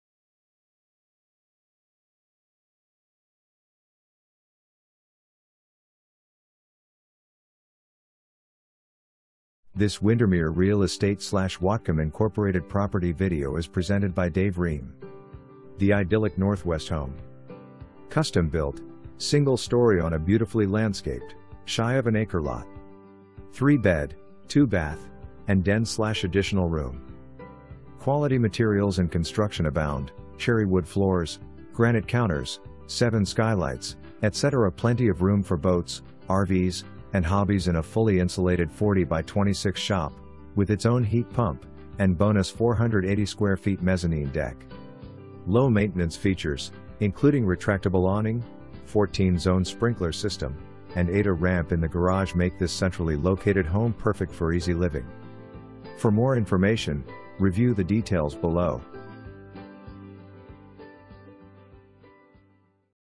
Windermere Real Estate/Whatcom, Inc. - 5715 Willow Springs Wy
The idyllic Northwest home. Custom built, single story on a beautifully landscaped, shy of an acre lot. 3-bed, 2-bath, and den/additional room. Quality materials & construction abound; Cherrywood floors, granite counters, 7-skylights, etc. Plenty of room for boats, RVs, and hobbies in a fully insulated 40x26 shop (with its own heat pump) and bonus 480sf mezzanine deck. Low maintenance features, including retractable awning, 14-zone sprinkler system, and ADA ramp in the garage make this centrally located home perfect for easy living.

2 full bath

Dave Rehm
Windermere Real Estate/Whatcom, Inc.